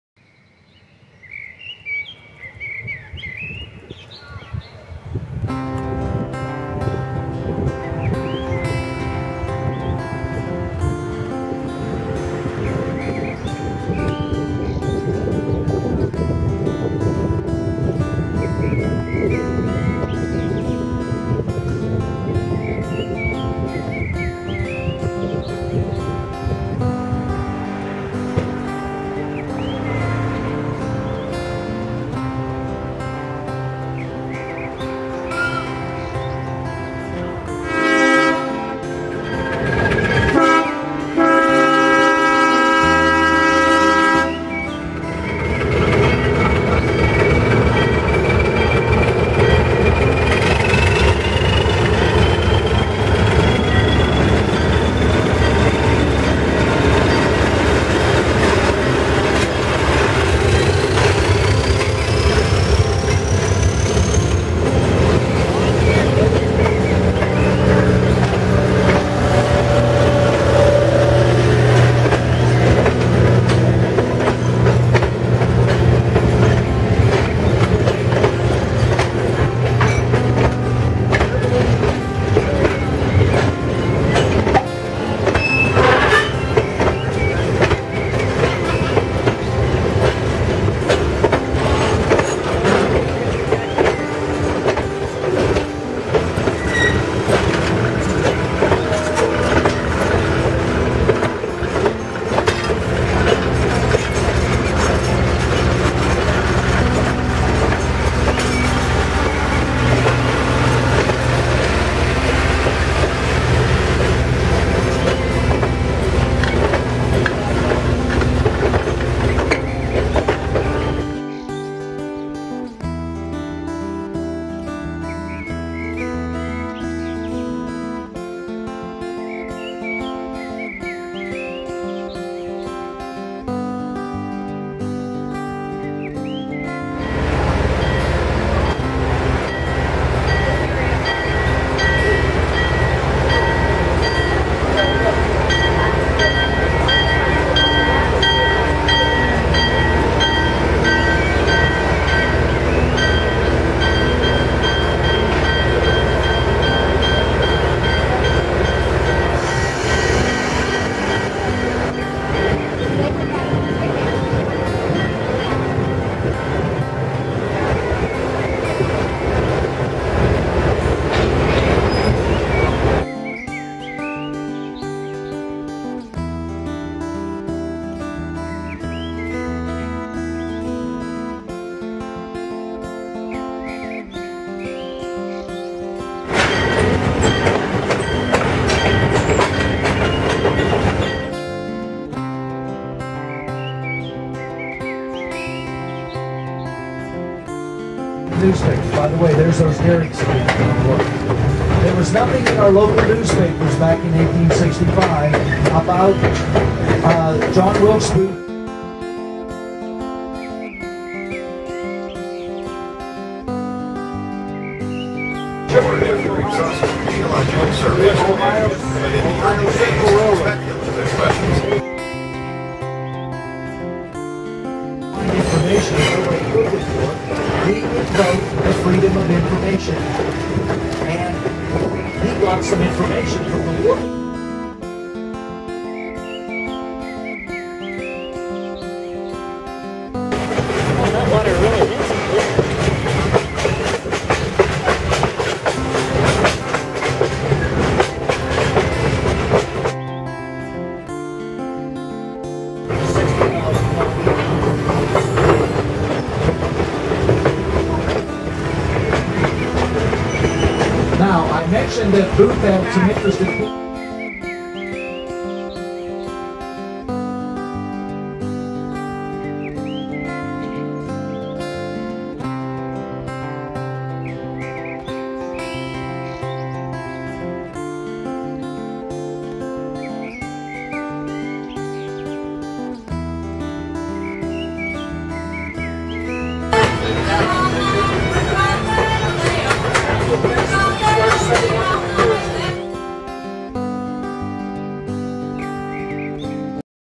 Oil Creek and Titusville Railroad Fall Leaves Tour
Here is some of my early footage with a small point and shoot.